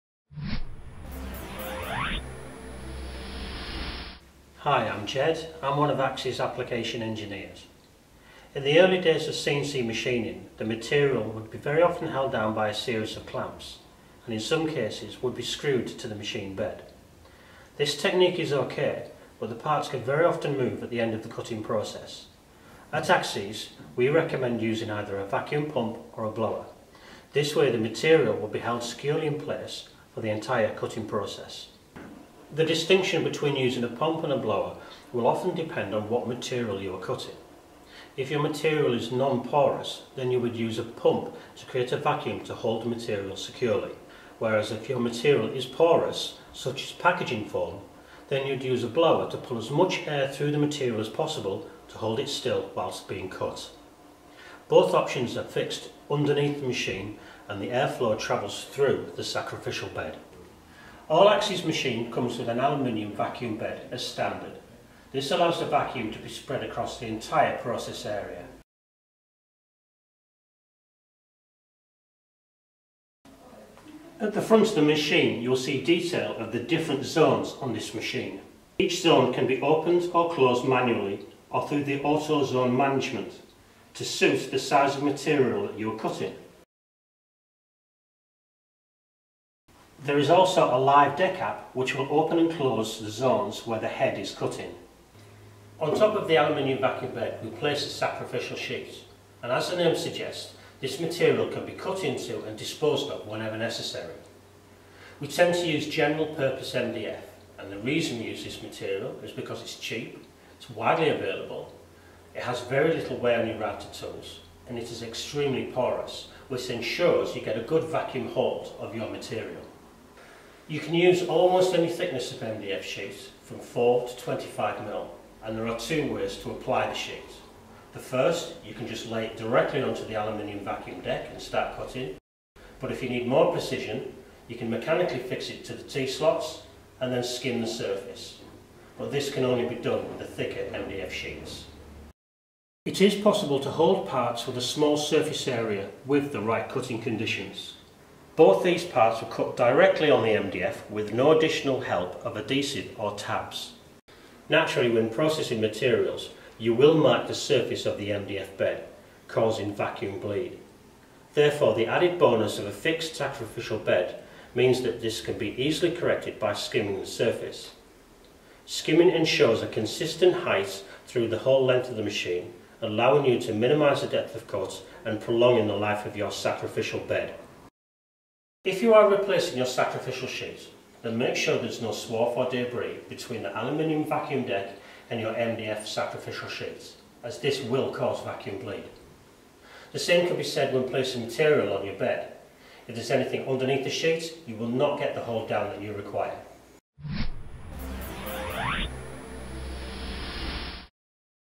The benefits of using a Vacuum Bed on an AXYZ CNC Router
During the early days of CNC routing, most machine held the material down with a variety of clamps, and in some cases the material would be screwed down to the bed. While this technique does provide material security it still allows movements of parts during the cutting process, which inevitably damage cut quality.

At AXYZ we recommend using a vacuum pump or blower. This way the material remains securely in place throughout the entire cutting process.

The distinction between using a pump or blower is often dependent on what type of material you are cutting. If the material is non-porous, then you would use a pump to create deep vacuum pressure. However, if your material is porous such as packaging foam, then the blower would be used to drag as much air through the material as possible.

Both of these options are plumbed into the underside of an AXYZ CNC Router and the air flows through the sacrificial bed.

All AXYZ CNC Routers come with an aluminum vacuum deck as standard. This allows the vacuum to spread across the entire process areas. The vacuum deck is split into several zones to allow vacuum zones to be closed or opened manually or through the auto zone management application. This application will open and close zones dependent on the live position of the cutting head, ensuring optimal vacuum hold down on all material being processed.

A sacrificial bed is placed on top of the aluminum vacuum deck. We recommend using general purpose MDF;

1. It's cheap
2. It's widely available
3. It produces very little wear on tooling.
4. Most importantly, it's extremely porous material allowing maximum air flow.

You can use almost any thickness of MDF, from 4mm up to 25mm. There are two ways to attach your MDF, firstly you can lay the material on the bed and use the vacuum hold down to keep it secure, or you can mechanically fix it in position, although this can only be done with thicker MDF.

Naturally, when processing material you will mark the surface of the MDF, unfortunately this will eventually cause vacuum bleed. With a mechanically fixed sacrificial bed, you can skim the surface to ensure a consistent bed height.

If you are replacing your sacrificial bed make sure there is no swarf or debris between the aluminum vacuum deck and your new piece of MDF. The same can be said when replacing the material for cutting.